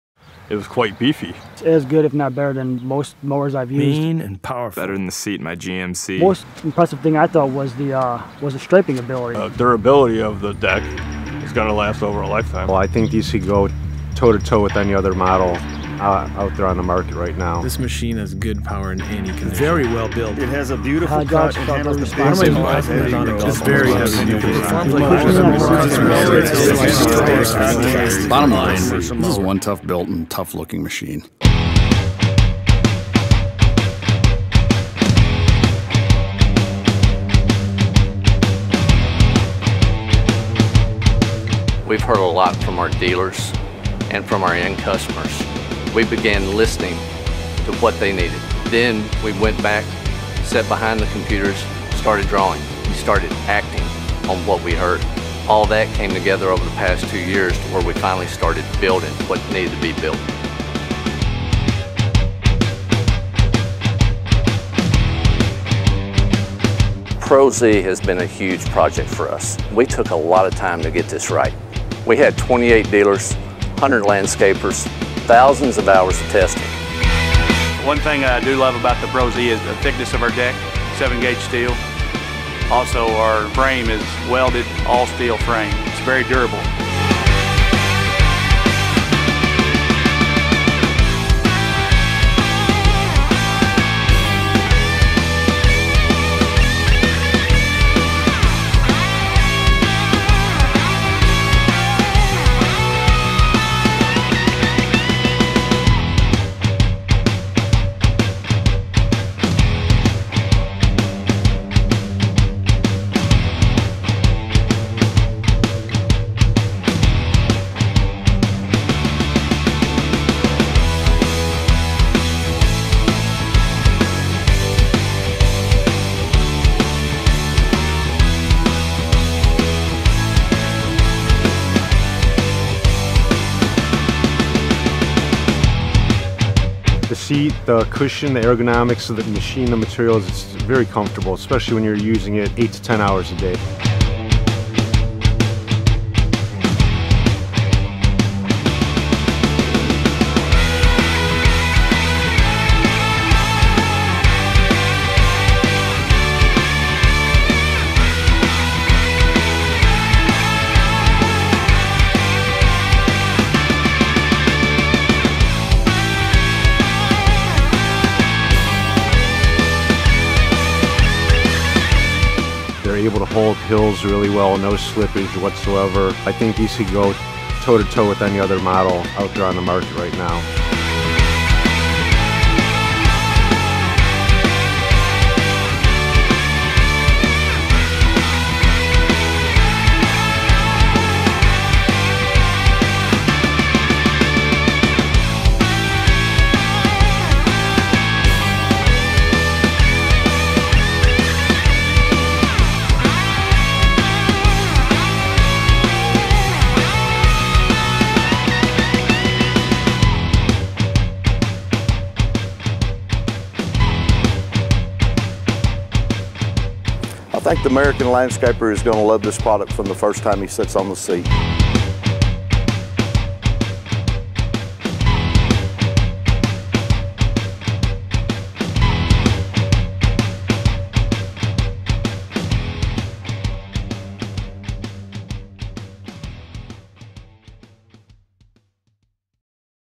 New PRO Z Series Cub Cadet Professional Zero Turn Mowers
Honest feedback from real Landscapers that informed the final design of the Cub Cadet Pro Z. The result is full complement of commercial zero-turn mowers with key focus on performance, durability and comfort.